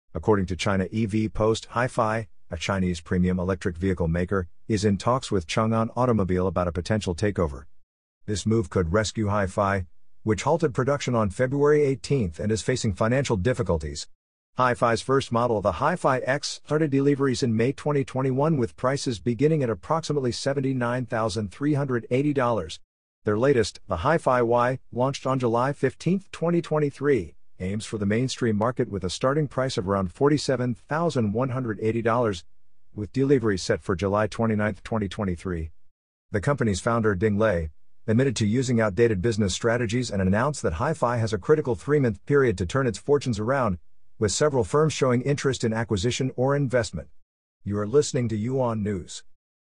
HiPhi in touch with Changan for takeover talks, report says
...Source Transcript: According to China EV Post, HiPhi, a Chinese premium Electric Vehicle maker, is in talks with Changan Automobile about a potential takeover. This move could rescue HiPhi, which halted production on February 18 and is facing financial difficulties. HiPhi's first model, the HiPhi X, started deliveries in May 2021 with prices beginning at approximately $79,380. Their latest, the HiPhi Y, launched on July 15, 2023, aims for the mainstream market with a starting price of around $47,180, with deliveries set for July 29, 2023. The company's founder, Ding Lei, admitted to using outdated business strategies and announced that HiPhi has a critical three-month period to turn its fortunes around, with several firms showing interest in acquisition or investment.